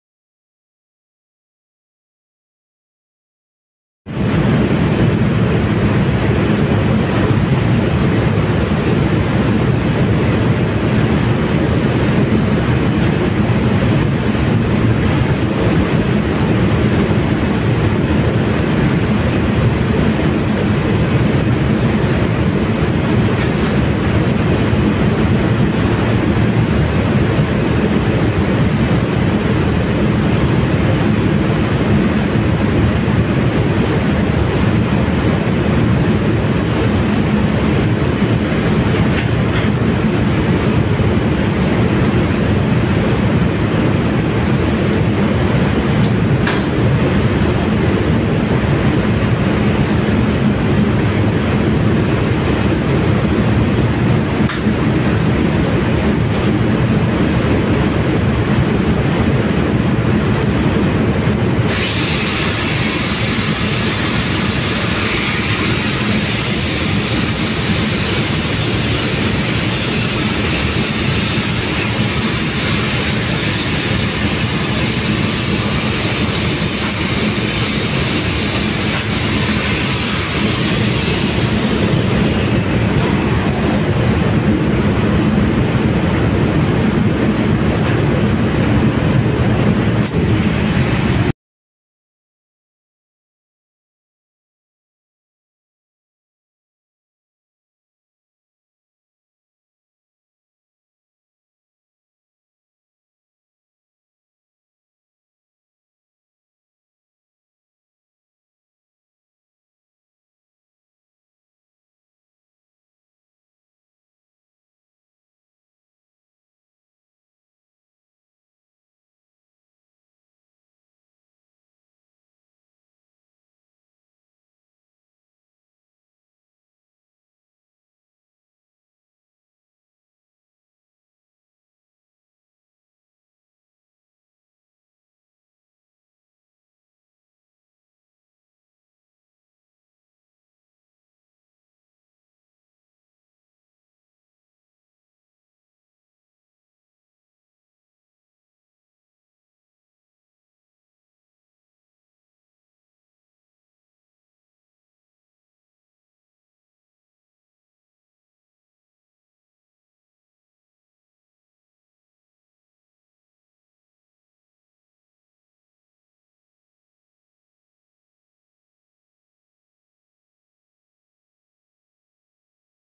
Trabaho ng Marino sa makina ng cruise ship
Hello beautiful people around the world, for today's video, ito po ang aking trabaho sa isang cruiseship.

I hope you enjoy watching my video. Thank you and God bless you.